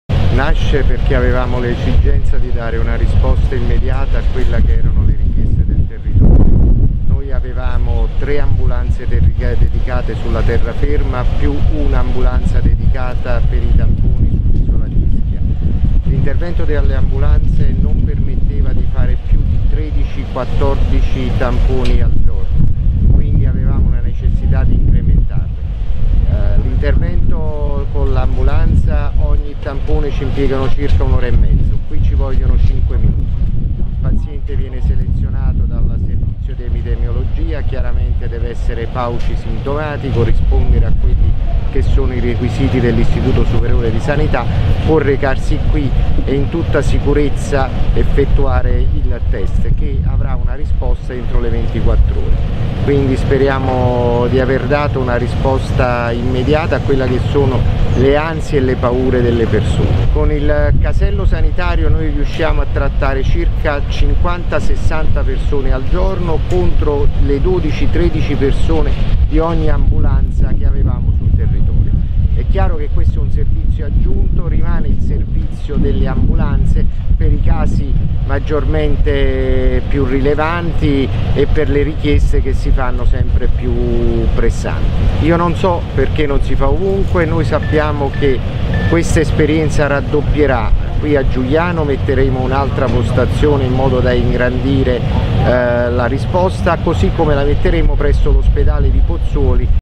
CV, a Giugliano parte il "casello tampone"
Coronavirus, a Giugliano parte il "casello tampone"

D'Amore: "Siamo già pronti a raddoppiare le postazioni"

 (Napoli). "Avevamo l'esigenza di dare una risposta immediata alle richieste del territorio. Le ambulanze garantivano al massimo 13/14 tamponi al giorno ed avevamo la necessità di incrementare. Con questo "casello" ci vogliono 5 minuti. Il paziente viene selezionato dal servizio di epidemiologia e  può recarsi qui, in tutta sicurezza, per effettuare il test. Così riusciamo ad effettuare circa 60 tamponi al giorno. Non so perchè non si faccia ovunque ma noi siamo già pronti a raddoppiare. Ci saranno altre due nuove postazioni: una a Giugliano ed una a Pozzuoli". Così il dr. Antonio d'Amore, direttore generale dell'Asl Na 2 nord. (Luca Leva/alanews)